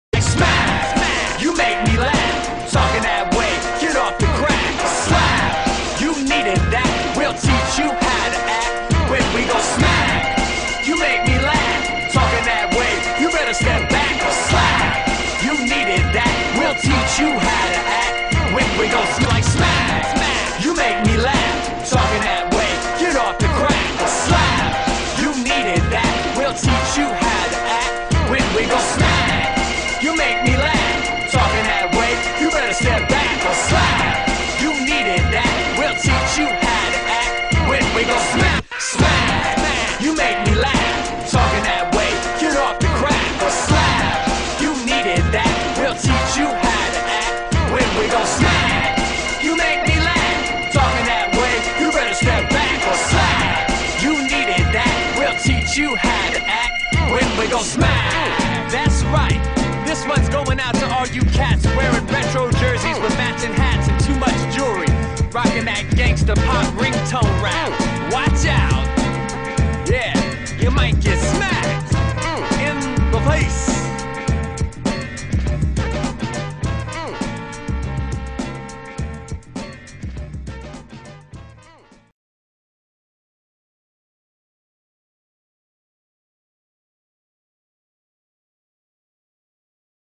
dog shaking off water in slow motion (420 fps)
our dog invents a new dance !